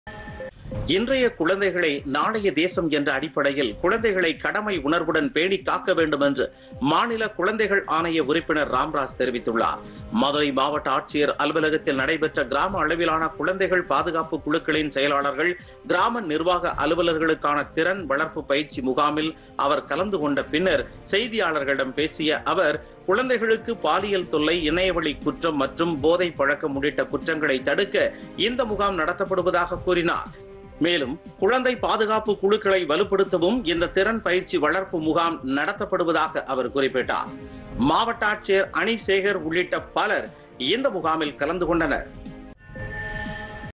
PodhigaiTamilNews பொதிகைசெய்திகள்
இன்றைய குழந்தைகளே நாளைய தேசம் என்ற அடிப்படையில் குழந்தைகளை கடமை உணர்வுடன் பேணிக்காக்க வேண்டும் என்று மாநில குழந்தைகள் ஆணைய உறுப்பினர் ராம்ராஜ் தெரிவித்துள்ளார்.